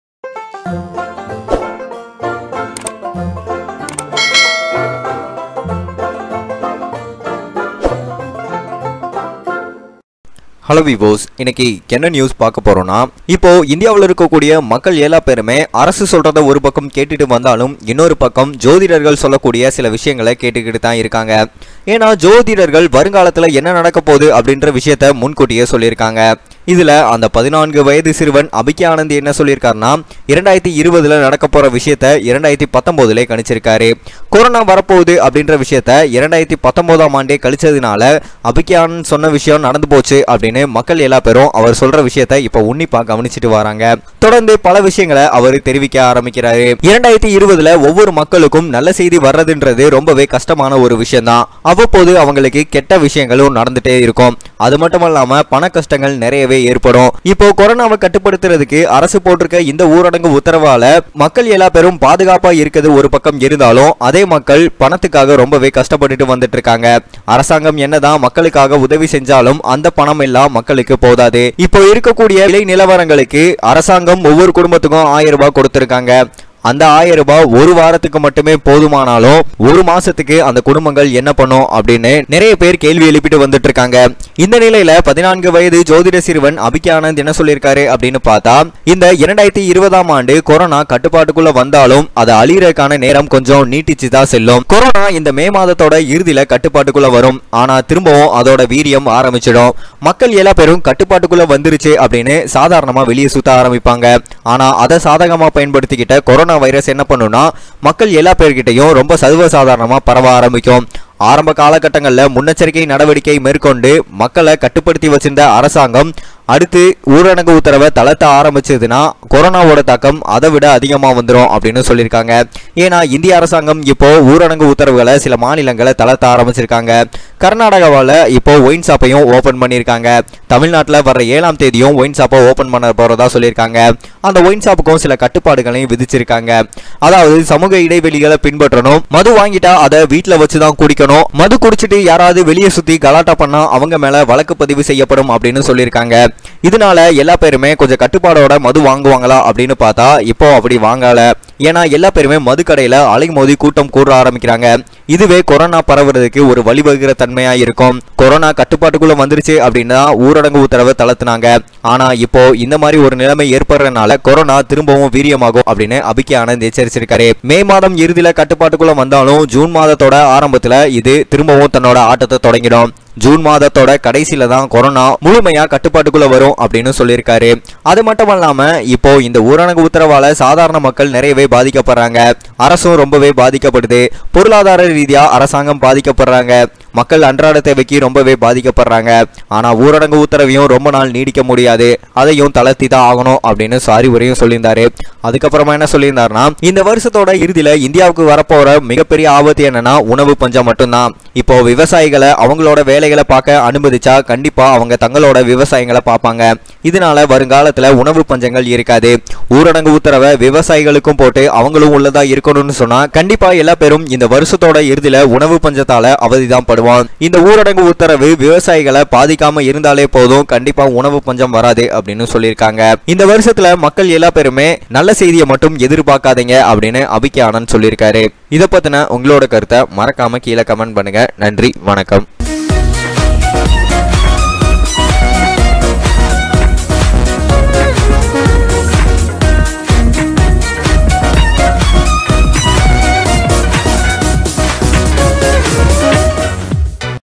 இந்த மாத இறுதியில் தமிழகத்திலும் நடக்க போகும் பேரழிவு; எச்சரிக்கை செய்ய ஆரம்பித்துள்ள ஜோதிட சிறுவன்
இந்த மாத இறுதியில் தமிழகத்திலும் நடக்க போகும் பேரழிவு; மீண்டும் எச்சரிக்கை செய்ய ஆரம்பித்துள்ள ஜோதிட சிறுவன்

teaching,scholarship, and research. - This Video has no negative impact on the original works (It would actually be positive for them)